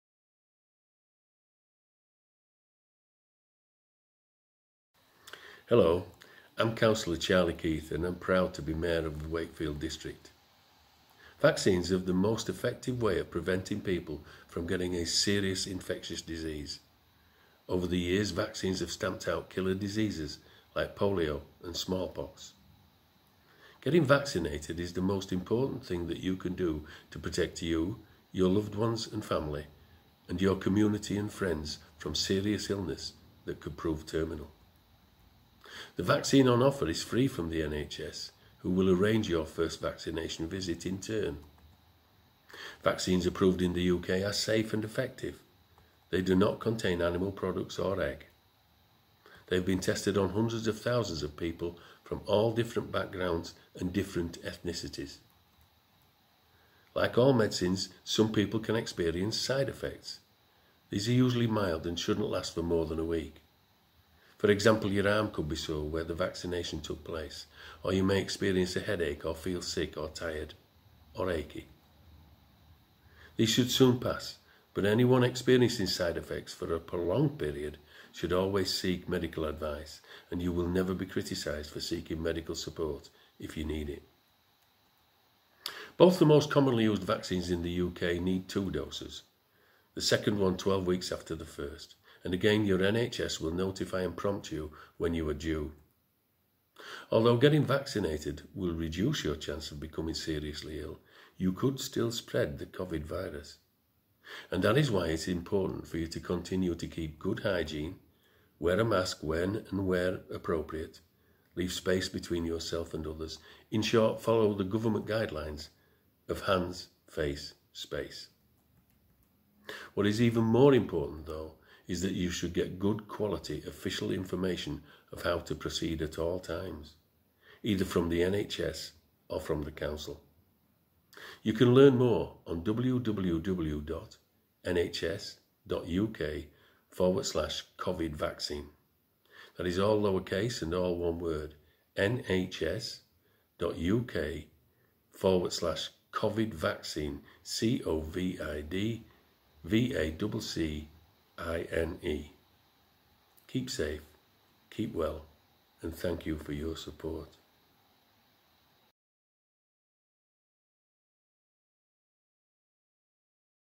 Mayor of Wakefield on Vaccination
The Mayor of Wakefield, Charlie Keith, talks about the importance of getting vaccinated.